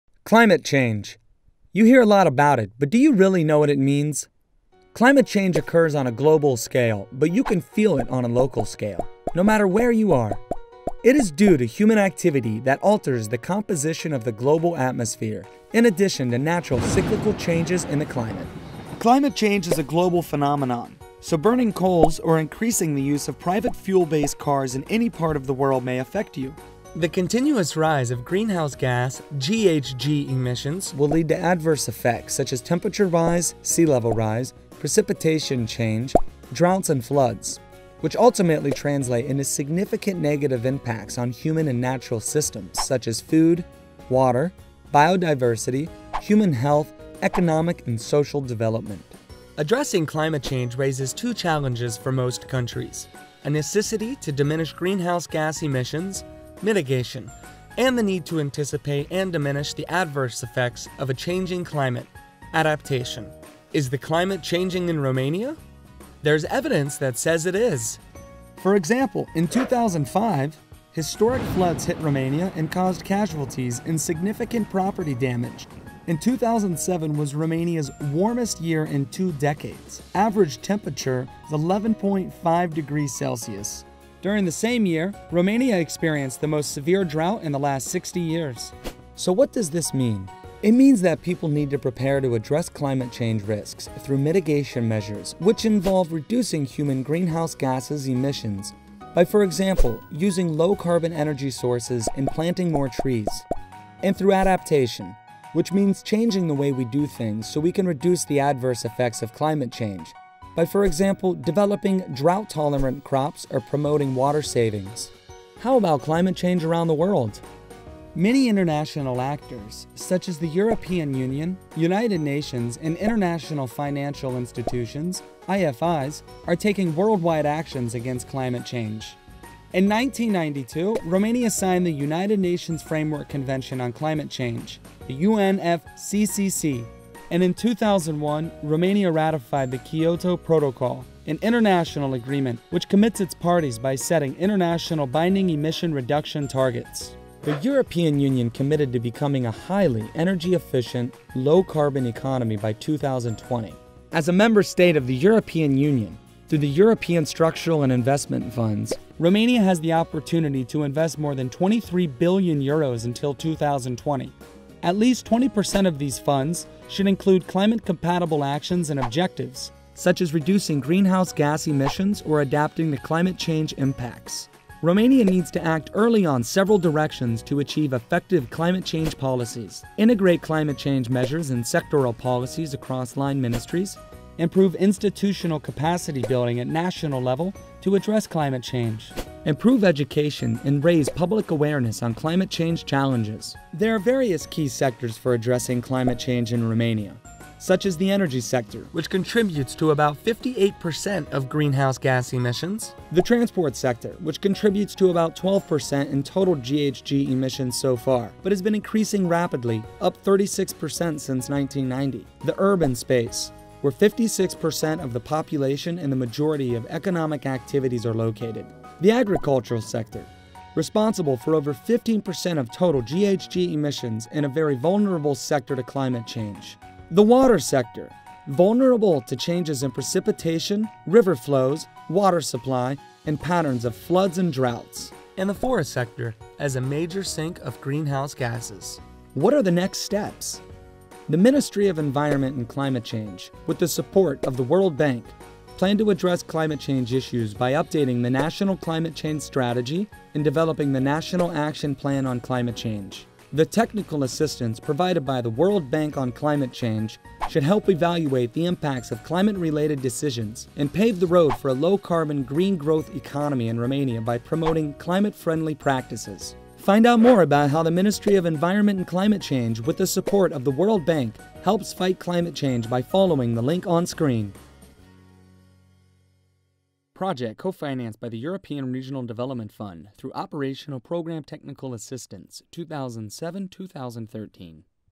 What is Climate Change? Mitigation and Adaptation in Romania
Romania is committed to moving towards a greener and more competitive low carbon economy that makes efficient use of resources and is resilient to climate risk. Therefore, the Government of Romania, through the Ministry of Environment and Climate Change, has requested the World Bank to provide advisory services to help meet this commitment. A program is implemented jointly by the World Bank and the Ministry, aiming to enable Romania reach the Europe 2020 objectives for fighting climate change and pursuing low carbon development.